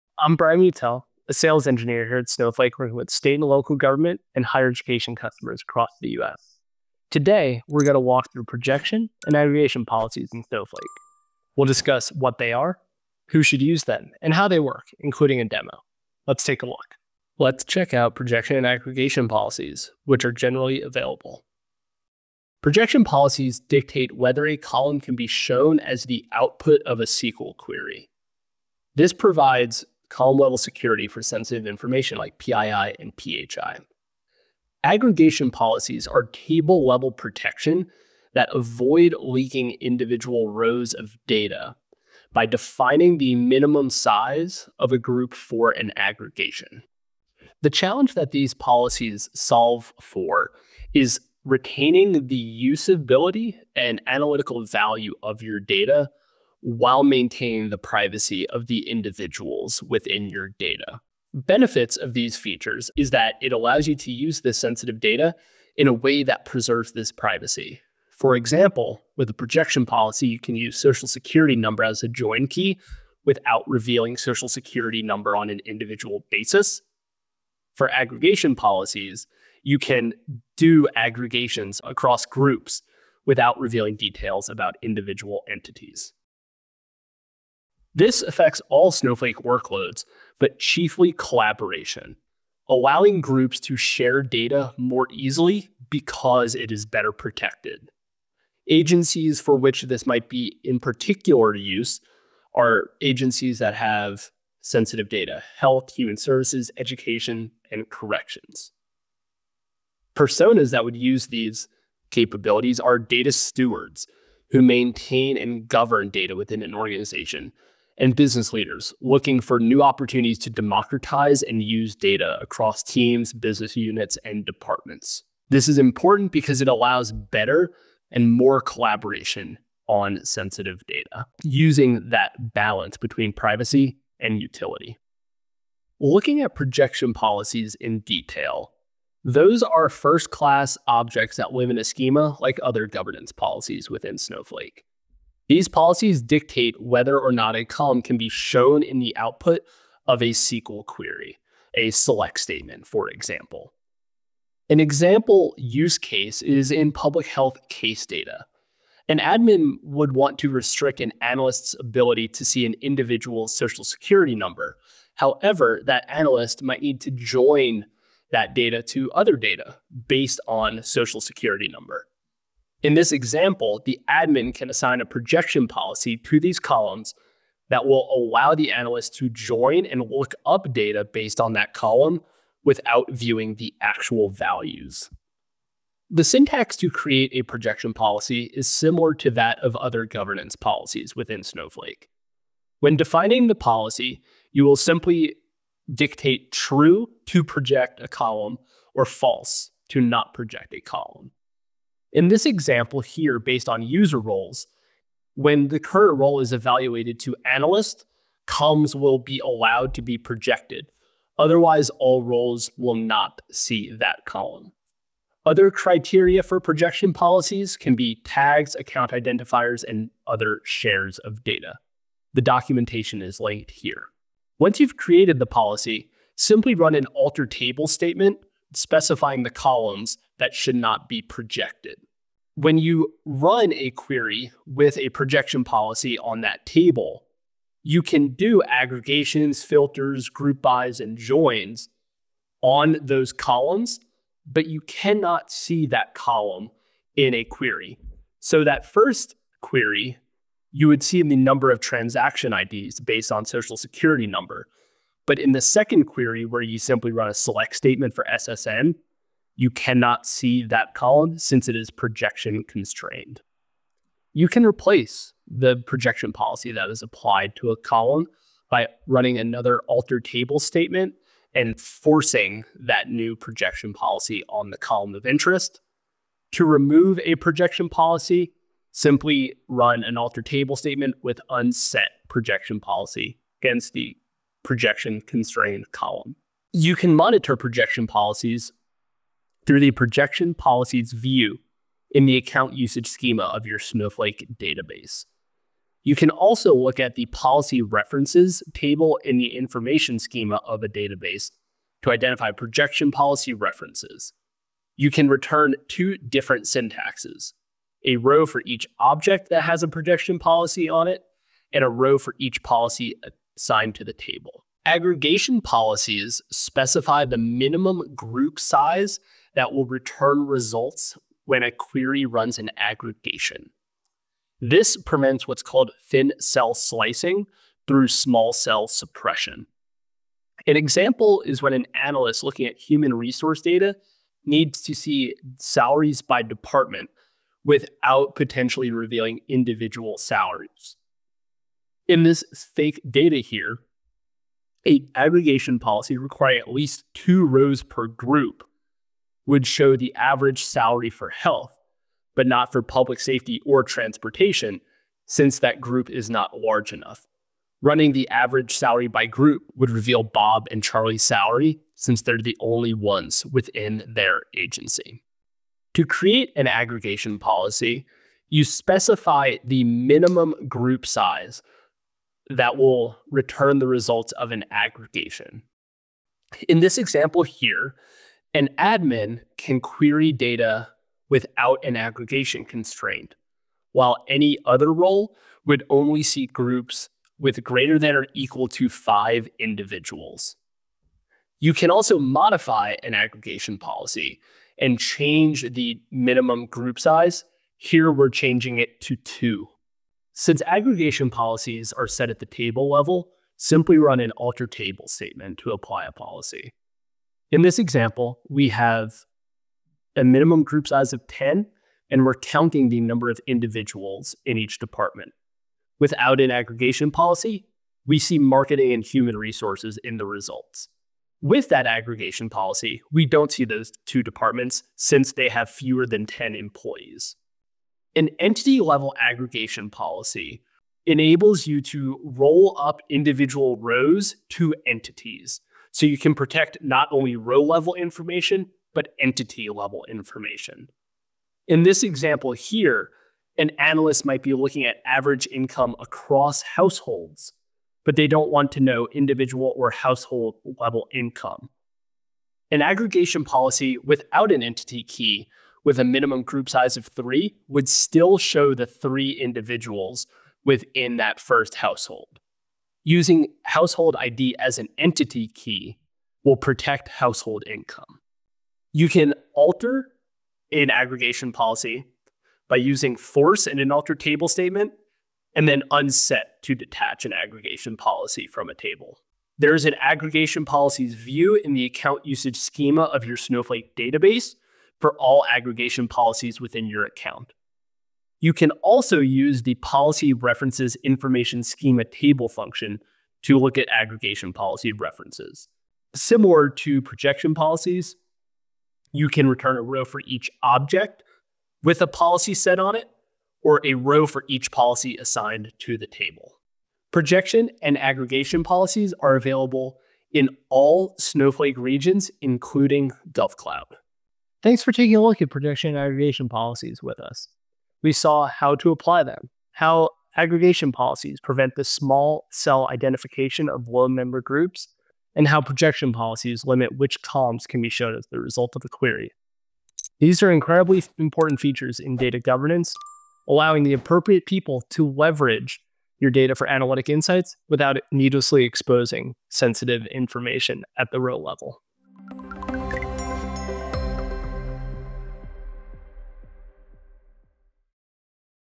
Snowflake Projection And Aggregation Policies: Enhance Data Security And Collaboration
In this video, we dive into Projection and Aggregation Policies in Snowflake, two powerful features that help you manage and secure sensitive data while unlocking new use cases.

What are Projection and Aggregation Policies? Projection Policies define whether a column can be included in the output of a SQL query. Aggregation Policies enforce that queries against aggregation-constrained tables must group data into minimum-sized chunks to prevent the return of individual records, helping protect privacy.

These policies are important for striking the perfect balance between privacy and utility. They enable:
  -- More collaboration on sensitive data
  -- Unlocking new use cases
  -- Sharing more in existing use cases